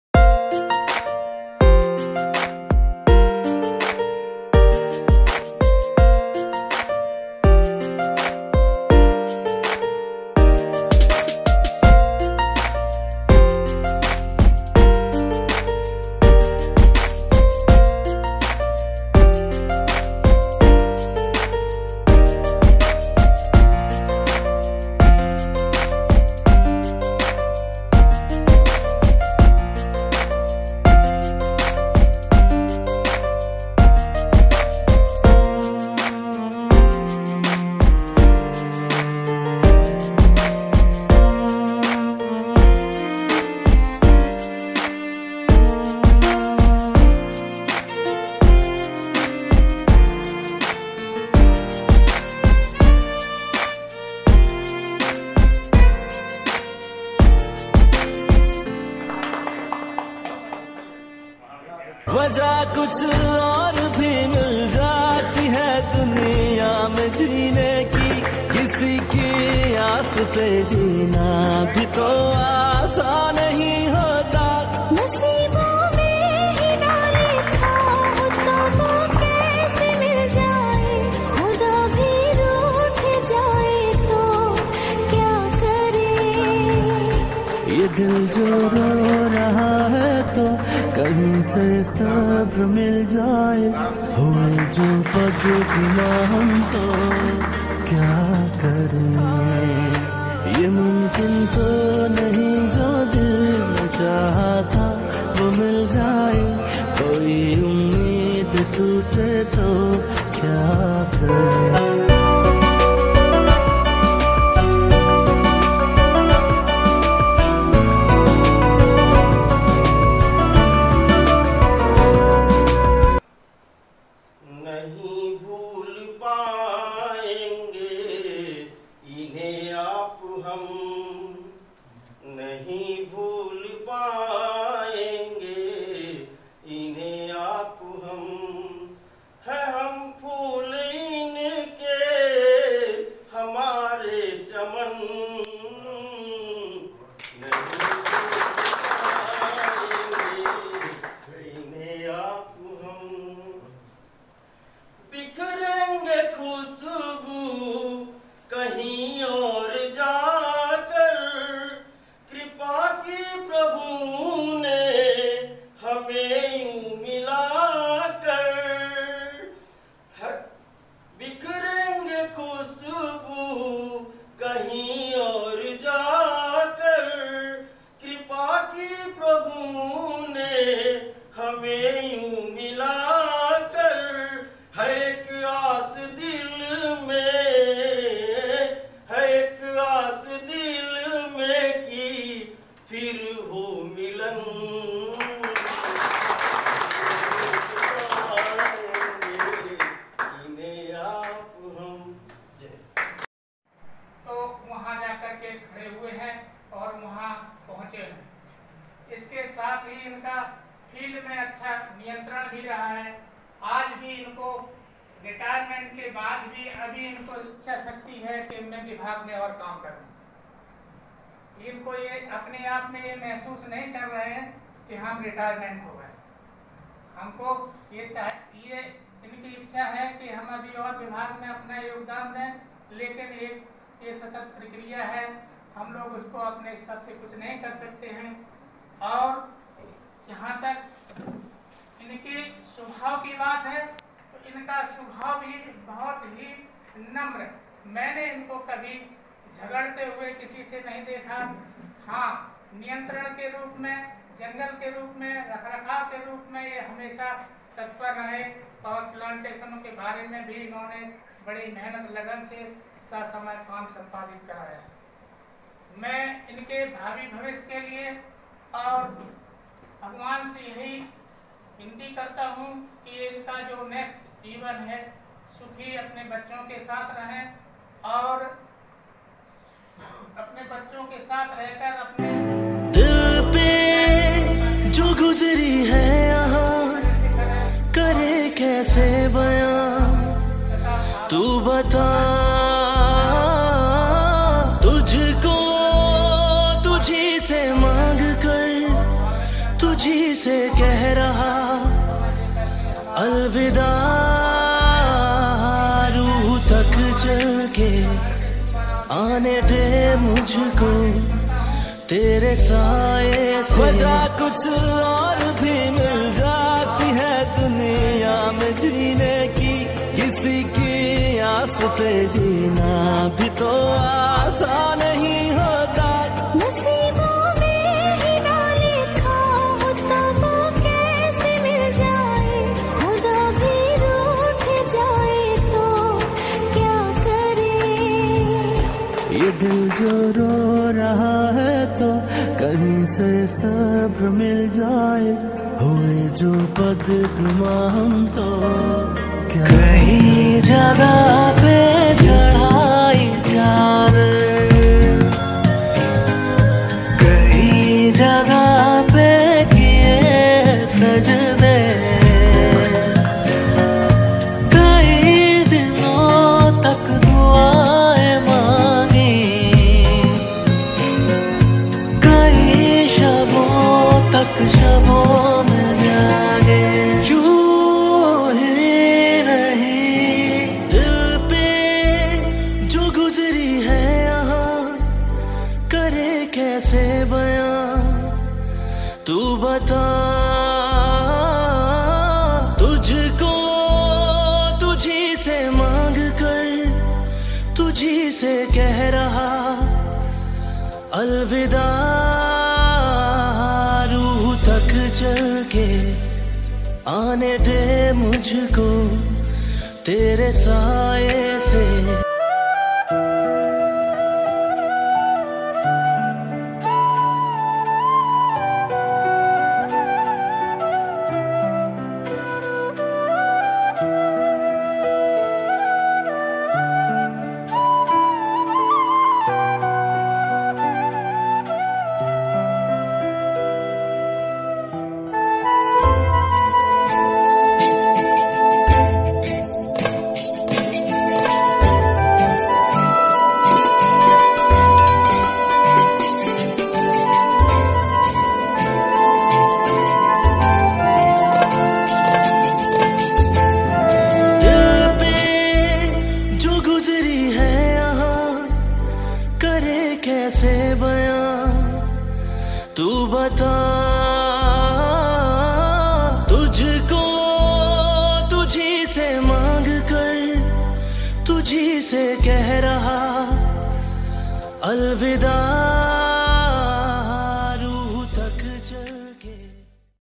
Ram Raj Chandravanshi farewell
Sub inspector Forest department
Bagharoo beat
Renukut